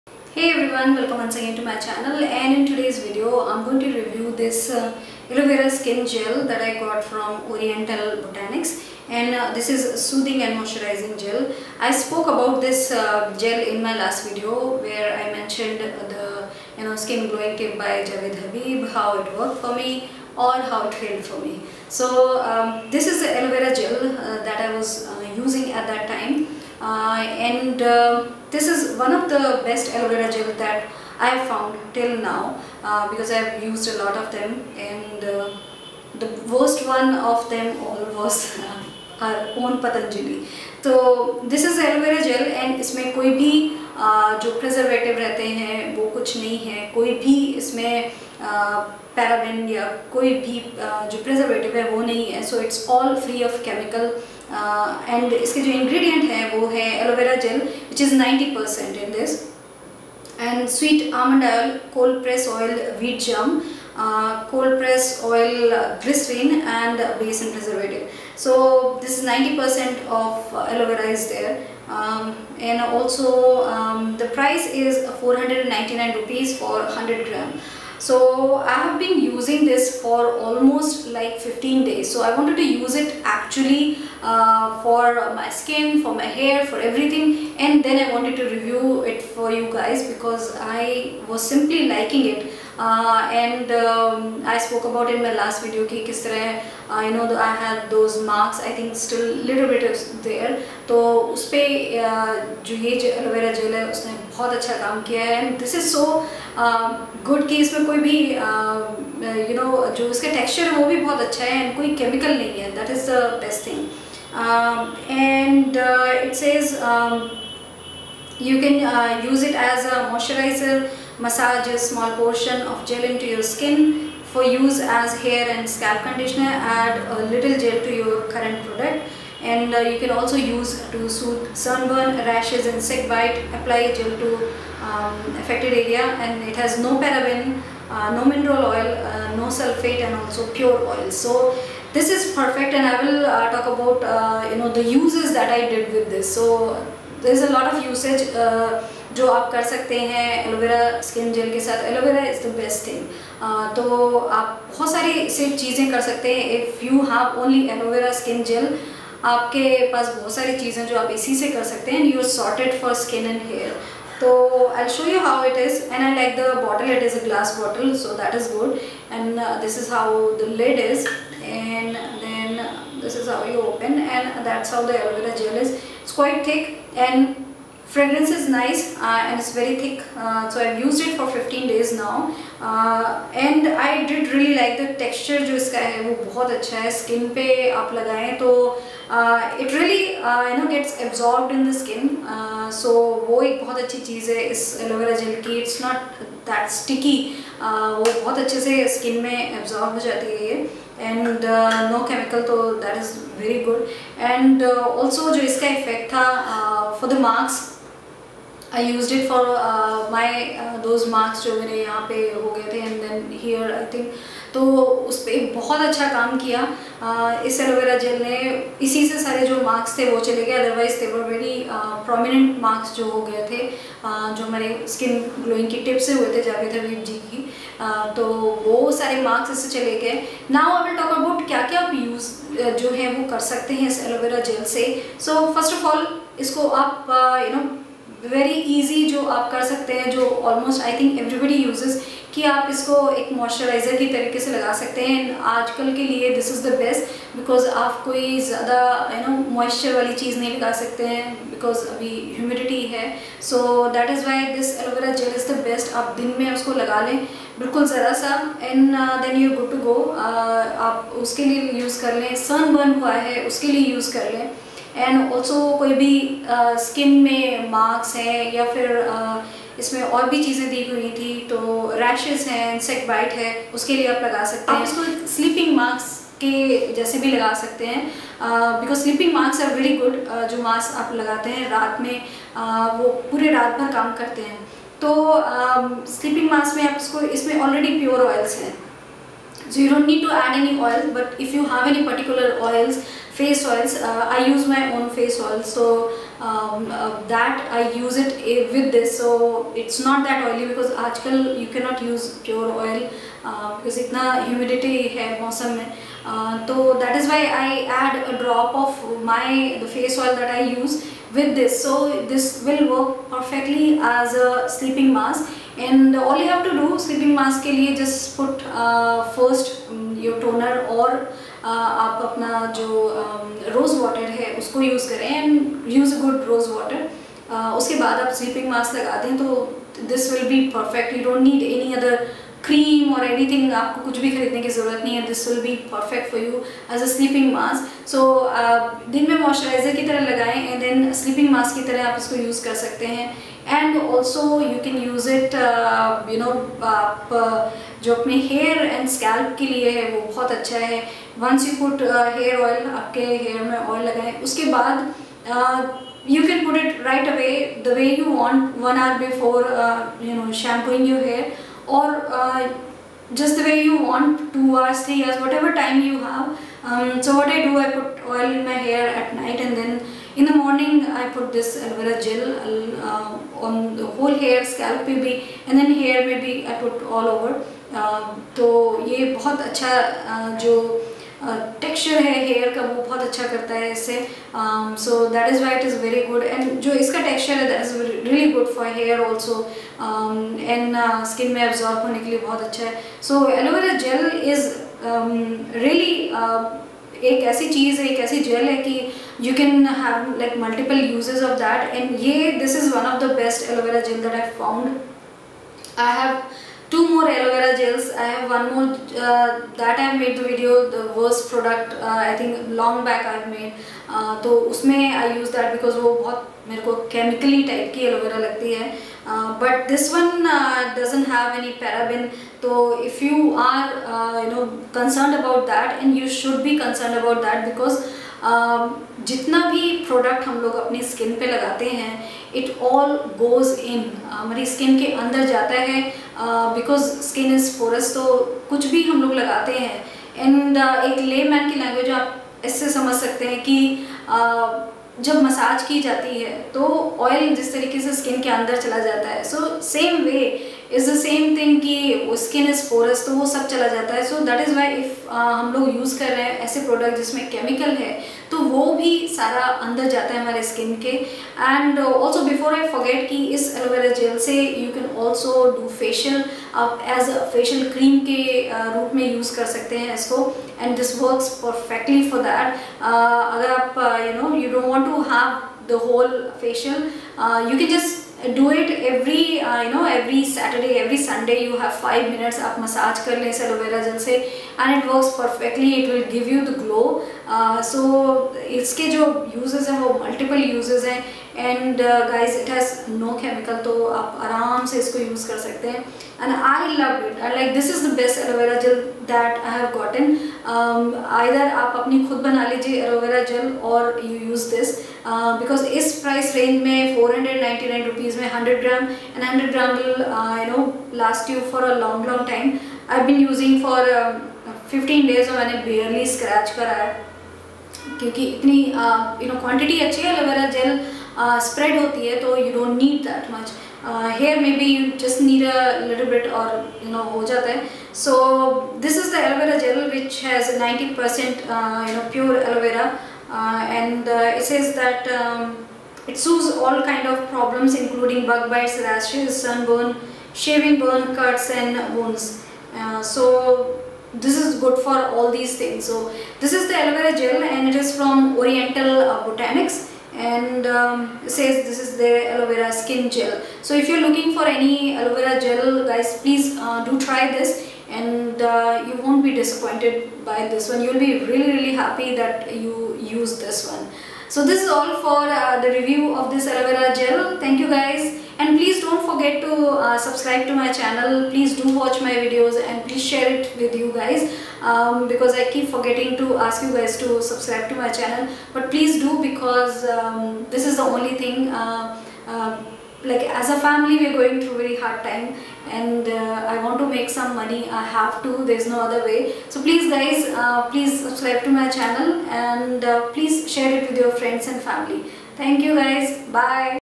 Oriental Botanics Aloe Vera Skin Gel Review
Hi everyone
I have reviewed Oriental Botanics Aloe Vera Gel in this video